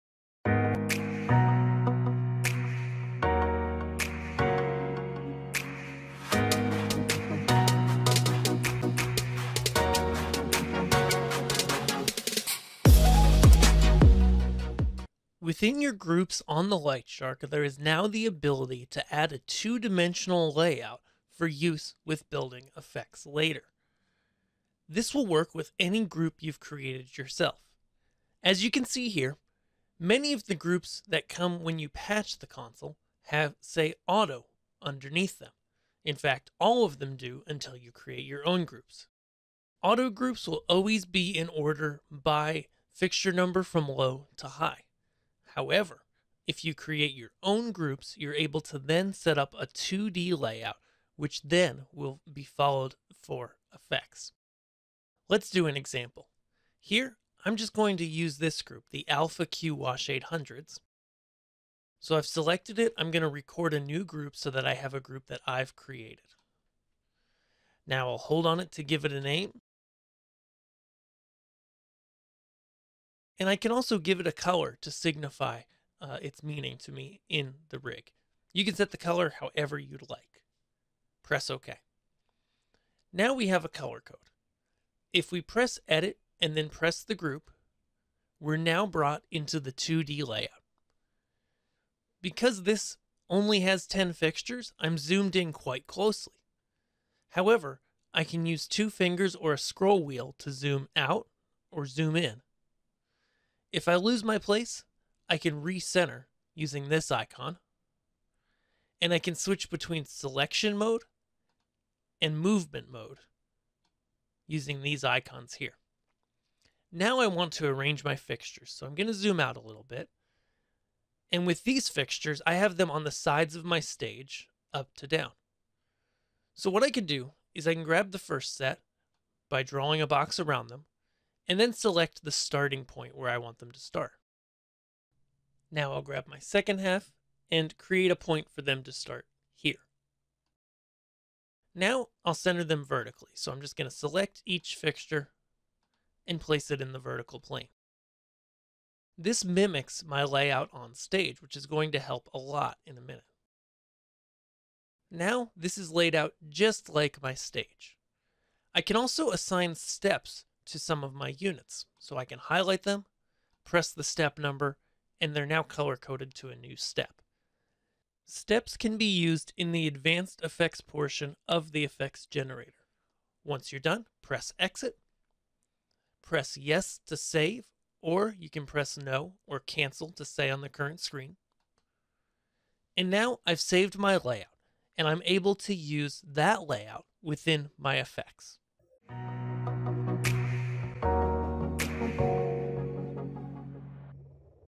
2D Layout for Groups in the LightShark
Use the 2D layout in the LightShark to make your groups respond to the new FX directions.  Here's how to get set up!